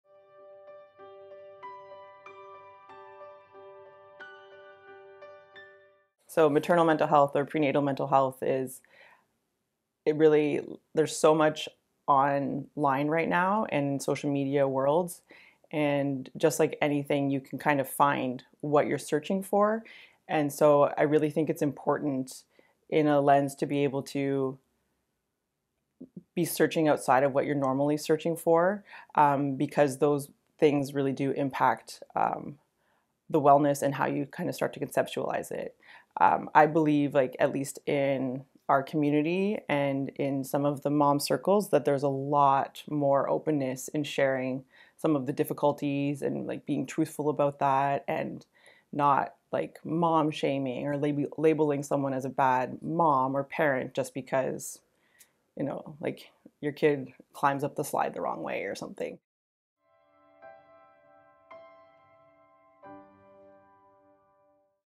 Maternal Mental Health | Laura Archer
Laura loves working with women, especially in the realm of maternal mental health. As a mother herself, Laura draws from personal experience as well as continuing education as a postpartum doula, childhood studies, and social work.

Laura's specialties include...
- Grief/Loss
- Maternal Mental Health
- Parenting
- Perinatal
- Self-Esteem

►To learn more about Laura and book your first session, head